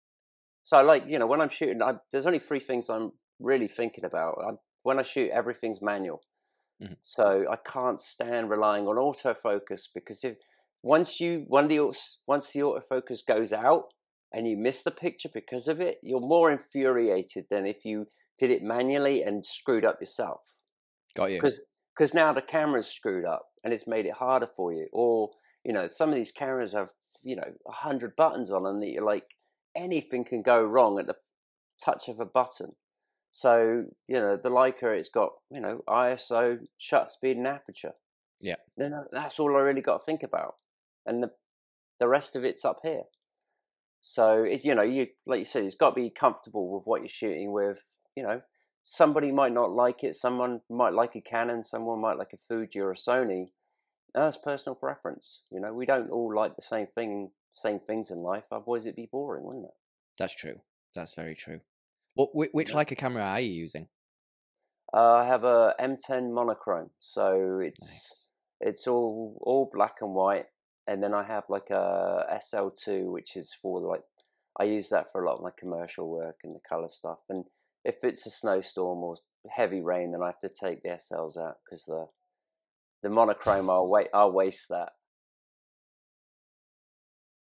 Phil Penman Shares Which Leica Cameras he Uses
On the latest episode of Inside The Photographer's Mind, Phil Penman shared what Leica cameras he uses, and why the Lecia system is his preferred choice.